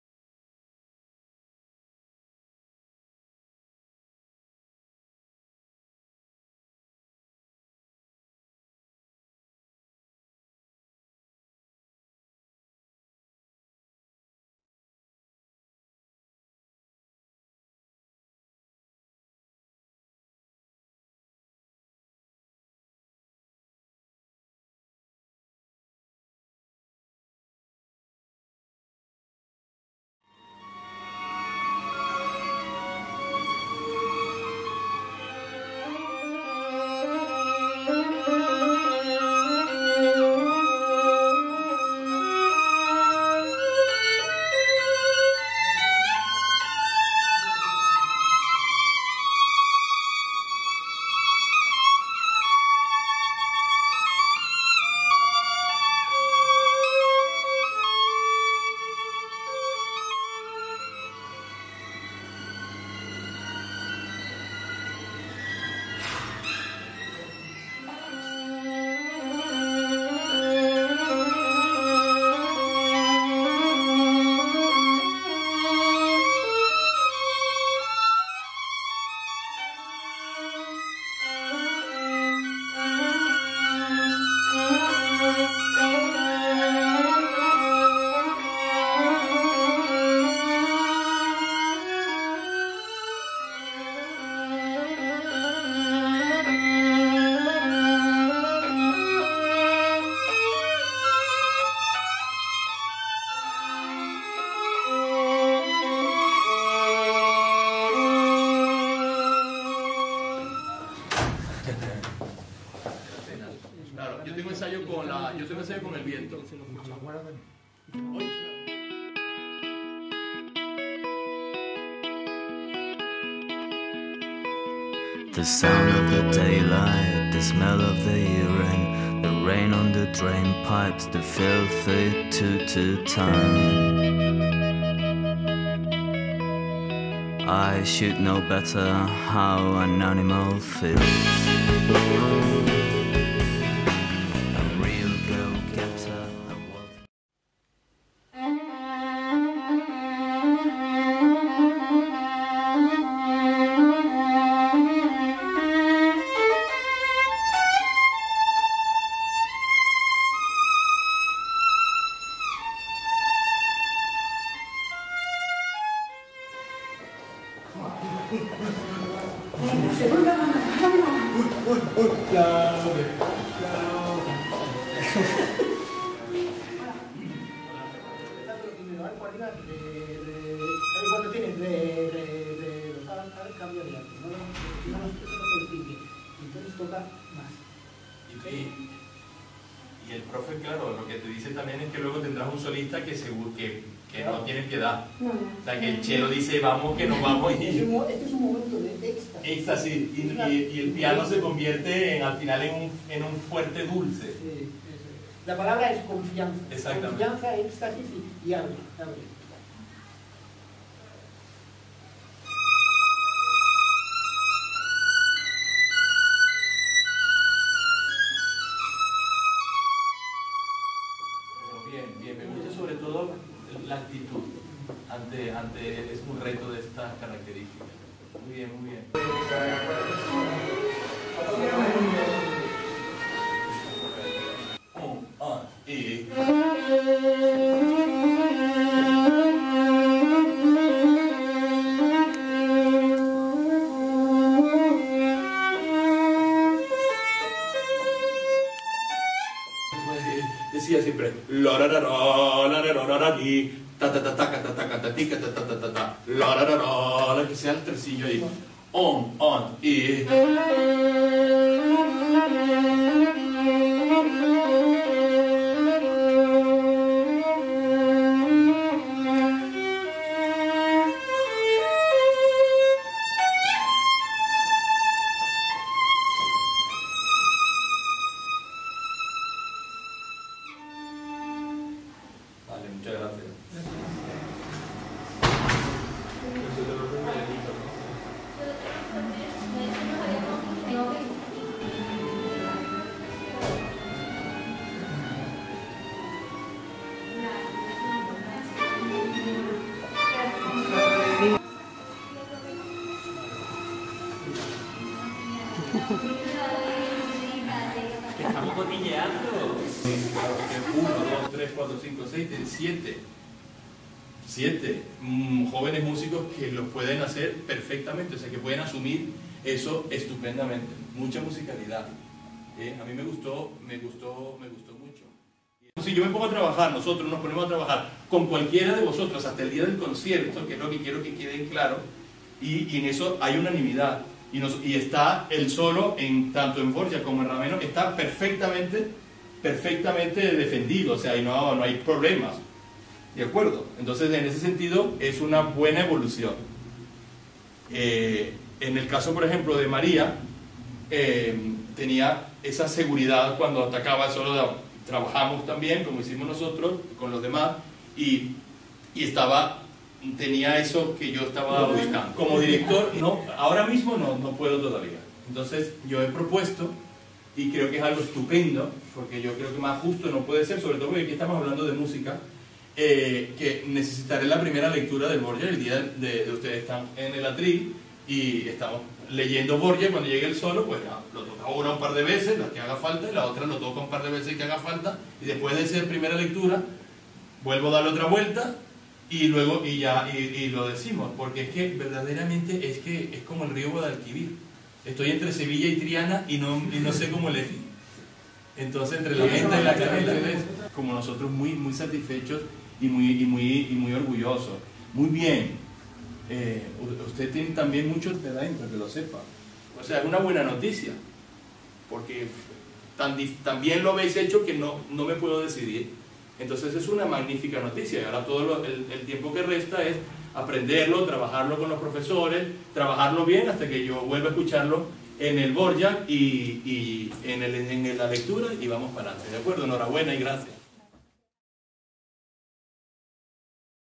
Orquesta Joven de Andalucía, OJA. Selección Concertino
Selección de Concertino durante el Encuentro de Primavera de la Orquesta Joven de Andalucía en marzo de 2016.

Realización: María Marí-Pérez